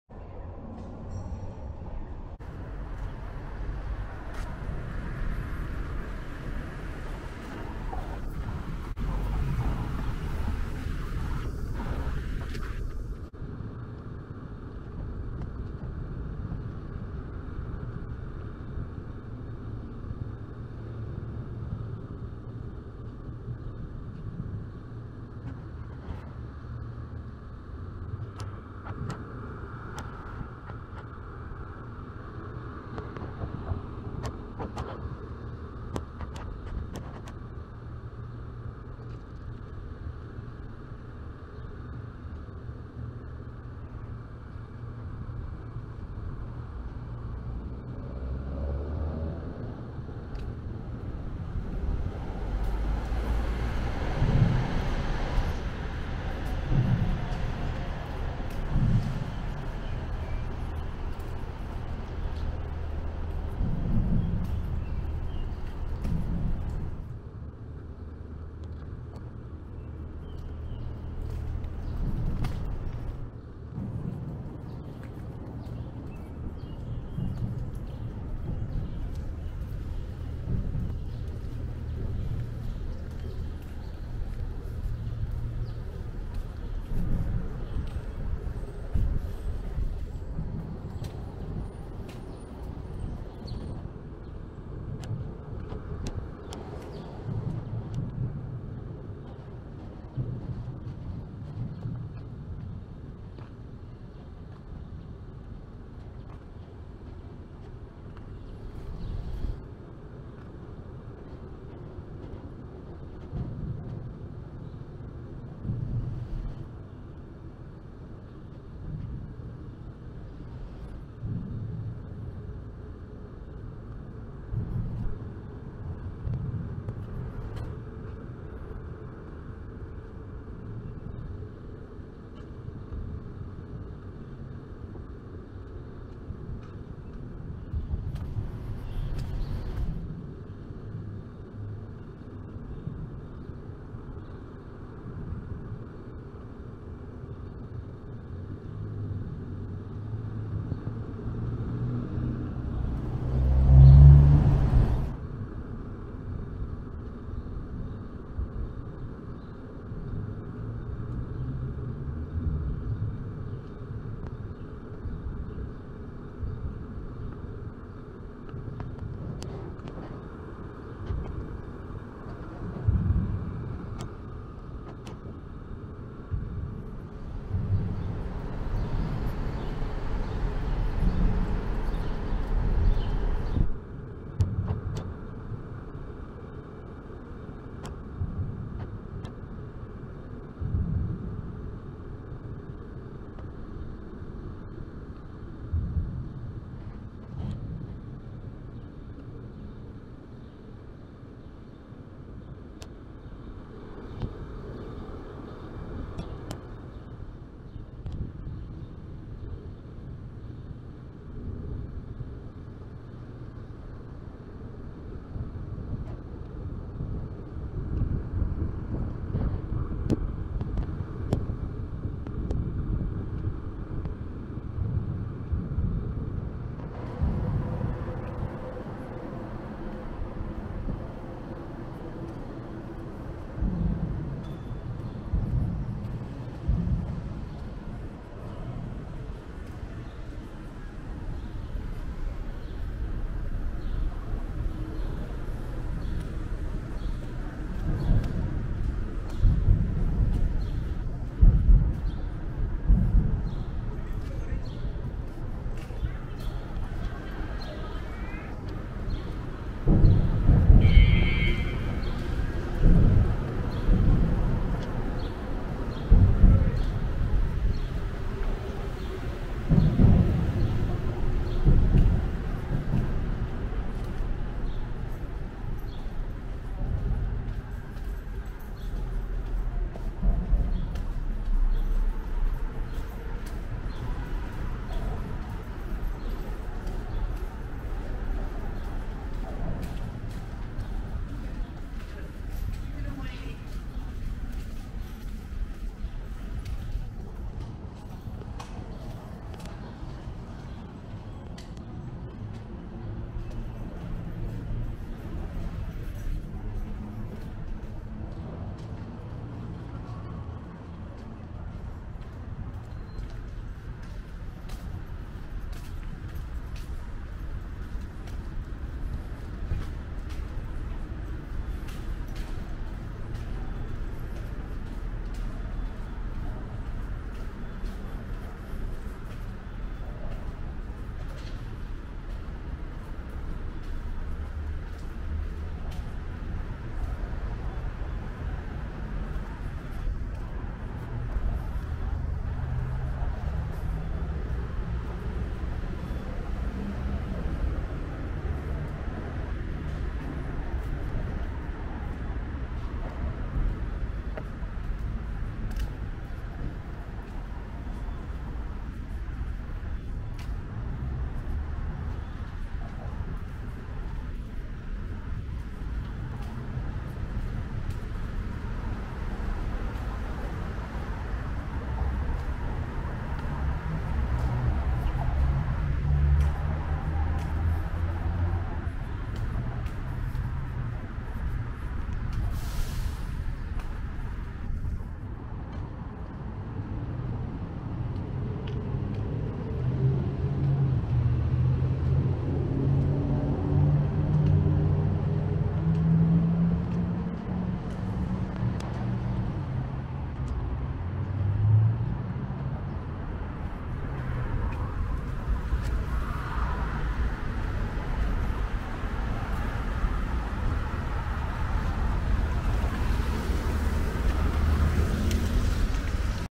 Walking and exploring under the 14th Street Viaduct in Hoboken, New Jersey, USA
An unnarrated walk tour exploring the area under the 14th Street Viaduct (under the bridge) in Hoboken, NJ. both sides of under the viaduct is also called Fourteenth Street (14th St). The walk starts just west of Madison St and ends where the viaduct ends at Willow Ave.

There are a lots of cool stuff to explore under this bridge. There is a park, a small soccer field, a basketball field, a plaza with events, such as markets, ect.

The 14th Street viaduct is a viaduct (a bridge) that connects Hoboken with Union City at Manhattan Ave, which sits on top of The Palisades cliff (that part of the cliff).